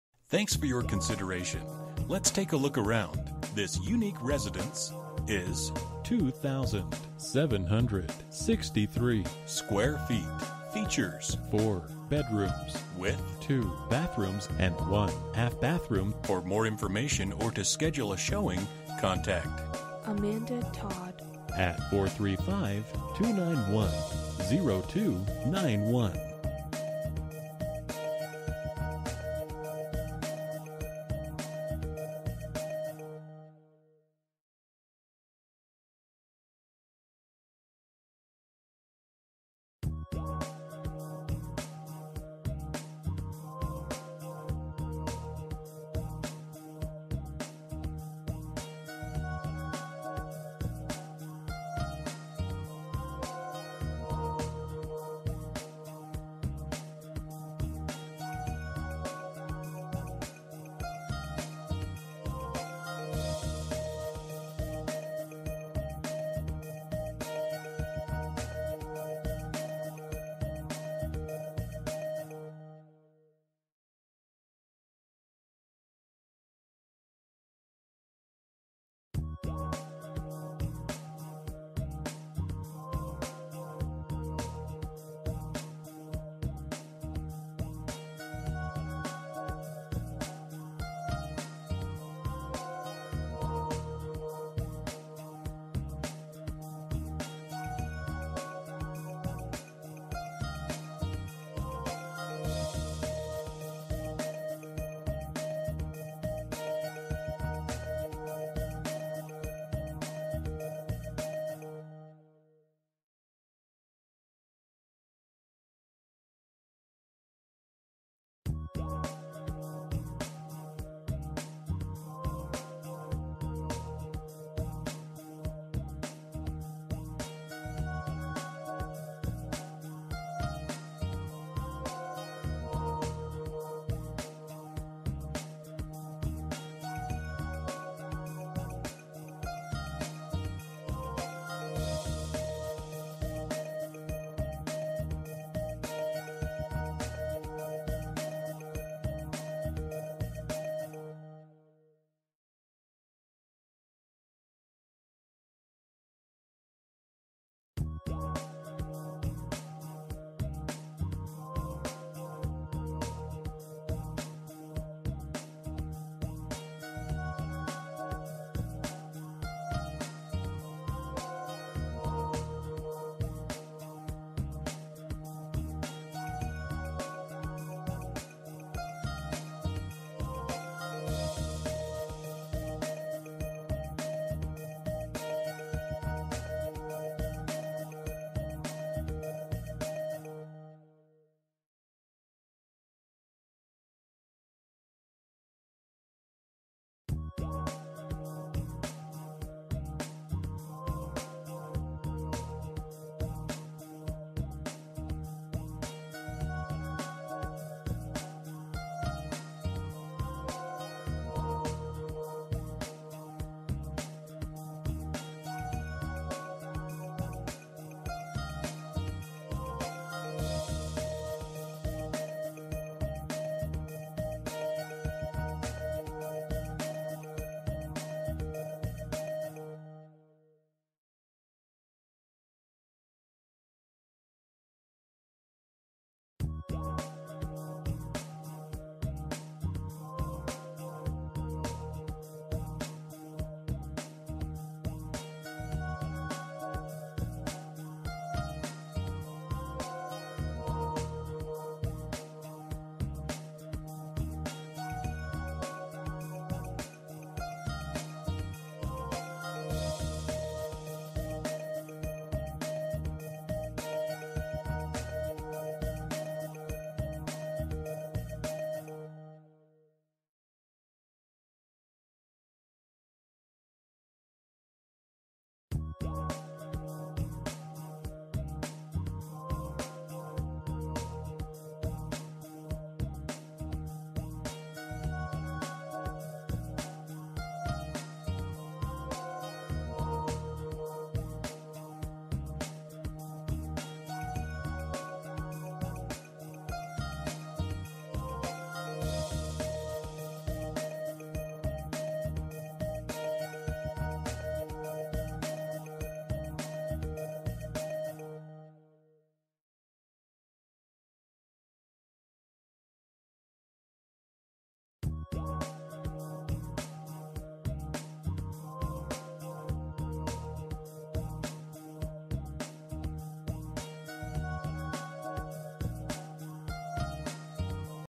Residential for sale - 1140 E Fort Pierce N 110, St George, UT 84790
This gorgeous St. George rambler has everything you could ask for! Located in a private, gated community, this home has luxurious features such as heated tile floors in the kitchen and beautiful master and guest baths. The kitchen has everything you need for cooking gourmet meals, including a 6-burner gas cooktop and oven, refrigerator and freezer, all professional Viking brand! The workshop comes

Bedrooms: 4
Bathrooms: 2 full and 1 half
Square Feet: 2763
Price: $779,000

For more information about this property, please contact AMANDA TODD at or . You can also text 5407310 to 67299.